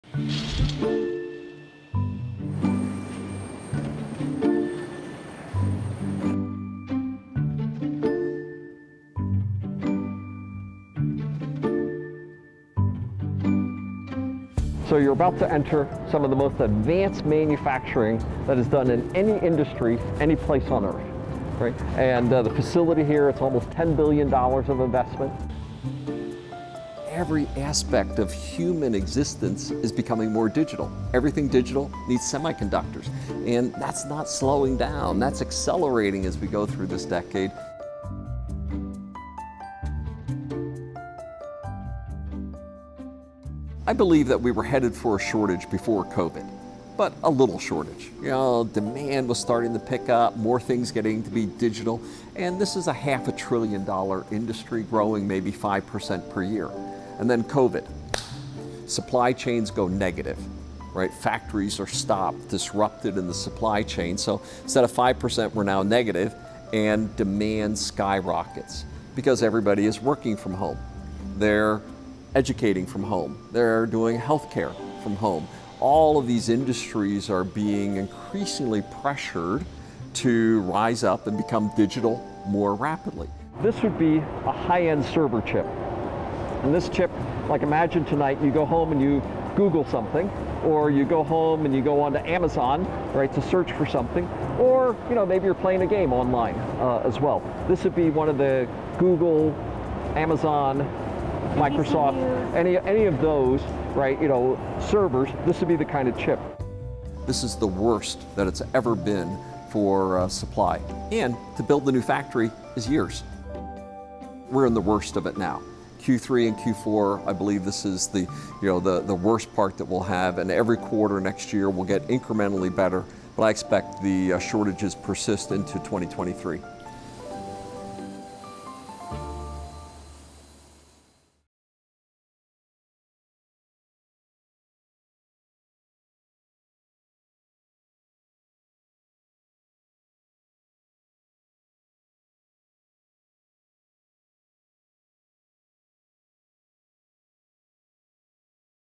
How Intel plan to help ease the global chip crisis - BBC News
Industries all over the world are suffering from a chip shortage.

Semiconductors are computer chips relied on by millions of products - such as cars, washing machines, and smartphones.

The BBC went behind the scenes at Intel to see what they are doing to try and boost supplies.